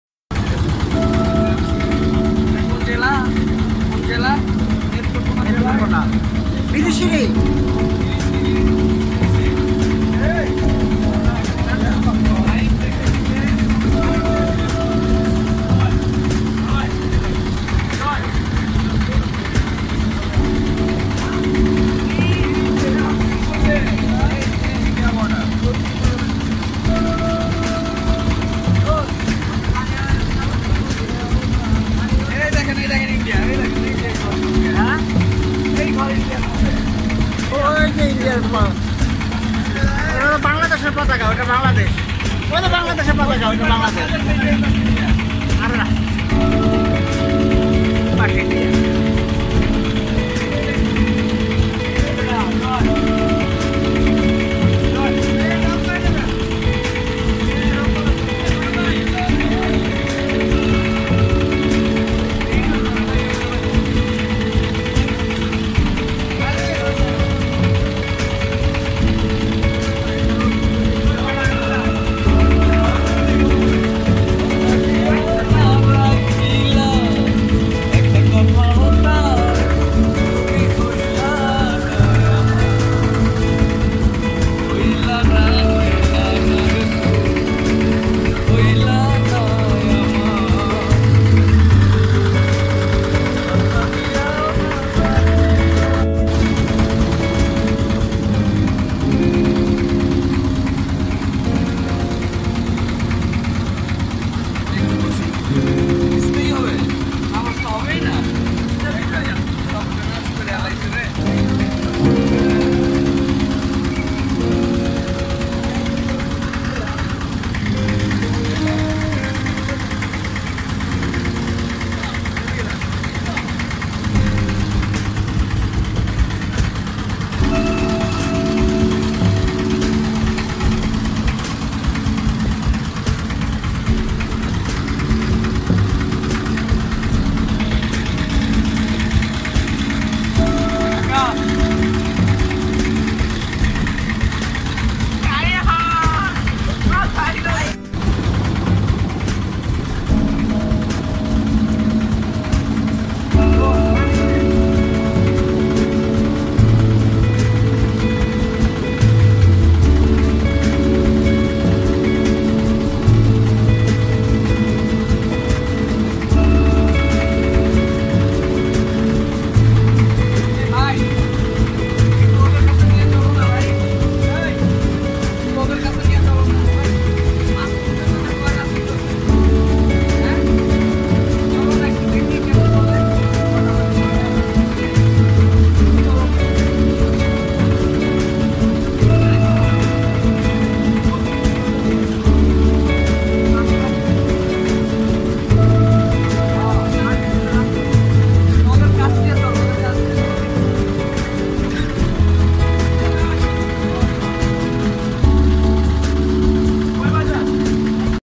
susun_duegapur_bijoynagar_Bangladesh
journey by boat at bijoy nagar near india border